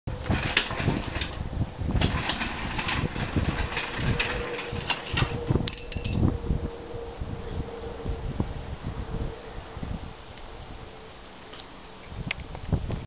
Scroungers CC
Pulling the front F body clip for the Ford truck.